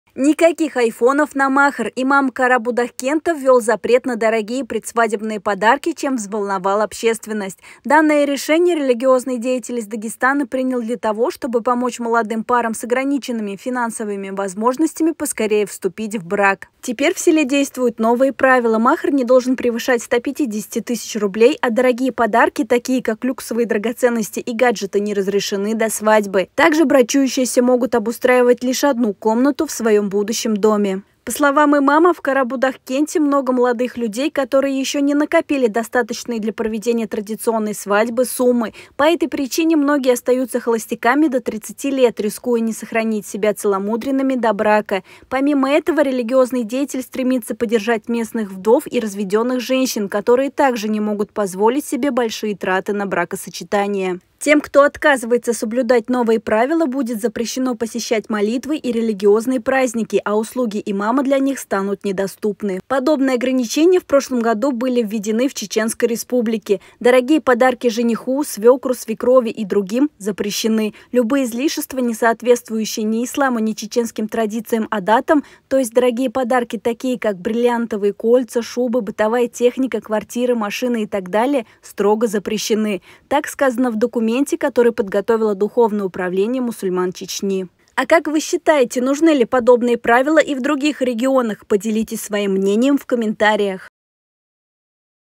Никаких айфонов на махр! Введён жесткий запрет!
Никаких айфонов на махр! Имам ввел запрет! Подробности — в нашем сюжете!